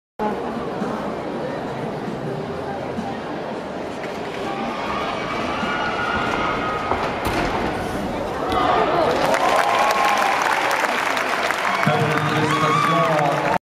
Chusovitina Oksana UZB VT1 qual 2013 Worlds
Qualified in 6th place with 14.750 - VT1: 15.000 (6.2)